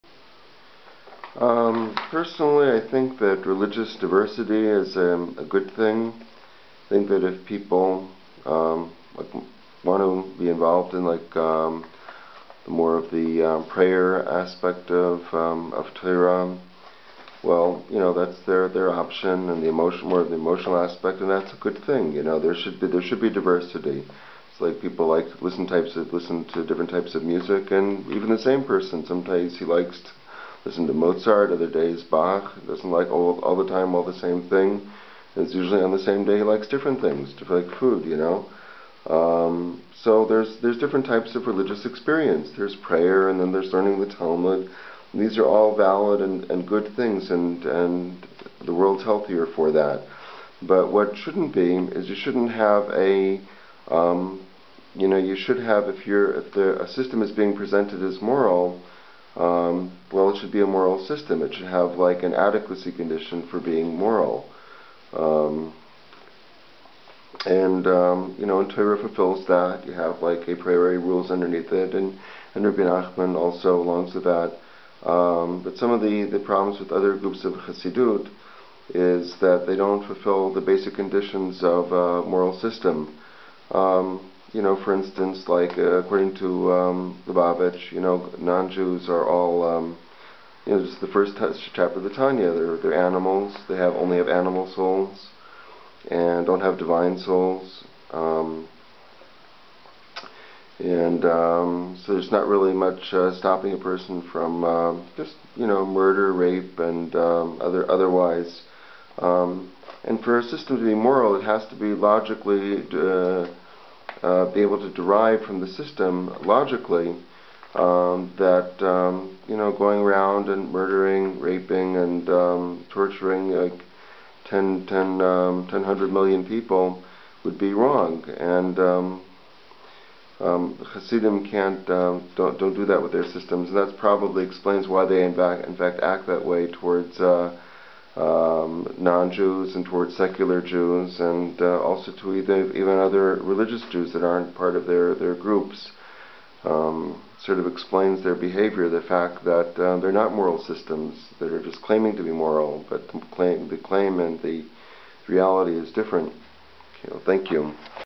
Religious Diversity and the Moral Integrity of Religious Systems
Summary:
The value of religious diversity is acknowledged, with various practices such as prayer and Talmud study each contributing uniquely to spiritual life. However, for a religious system to be considered truly moral, it must uphold genuine ethical standards. This involves having a consistent moral framework that logically prohibits actions like murder, rape, and torture. Some religious groups fail to meet these moral criteria, as evidenced by teachings that dehumanize non-Jews and justify unethical behavior. Such inconsistencies between claimed and actual moral values can explain problematic behaviors towards non-Jews, secular Jews, and other religious groups.

סיכום:
ערך גיוון דתי מוכר, כאשר פרקטיקות שונות כגון תפילה ולימוד תלמוד תורמות כל אחת באופן ייחודי לחיים הרוחניים. עם זאת, כדי שמערכת דתית תחשב לאמיתית ומוסרית, היא חייבת לשמור על עקרונות אתיים אמיתיים. זה כולל קיום מסגרת מוסרית עקבית האוסרת פעולות כמו רצח, אונס ועינויים. קבוצות דתיות מסוימות נכשלות לעמוד בקריטריונים מוסריים אלה, כפי שמעיד על כך לימוד שמפחית את ערכם של שאינם יהודים ומצדיק התנהגות לא מוסרית. חוסר התאמה בין הערכים המוצהרים לערכים האמיתיים יכול להסביר התנהגויות בעייתיות כלפי שאינם יהודים, יהודים חילוניים וקבוצות דתיות אחרות.

Personally, I believe that religious diversity is valuable. Just as people enjoy different types of music or food, religious practices can vary widely. Some individuals might focus more on the emotional aspects of prayer, while others might be drawn to the intellectual study of Talmud. Both are valid and contribute to a richer, more varied spiritual experience.

However, a religious system presented as moral should genuinely adhere to moral standards. It must meet certain criteria to be considered truly moral, which includes having a consistent, ethical framework. For instance, some groups fail to meet these moral standards. For example, certain teachings, such as those found in the first chapter of the Tanya, dehumanize non-Jews by describing them as having only animal souls, not divine ones. This lack of a moral foundation can lead to justifications for unethical behavior, including violence and exploitation.

A genuine moral system should logically derive that actions like murder, rape, and torture are wrong. Systems that do not establish this basis for morality may fail to prevent such behaviors and may even rationalize them. This discrepancy often explains why some groups act in ways that contradict their claimed moral values, especially towards non-Jews, secular Jews, and even other religious Jews outside their own group.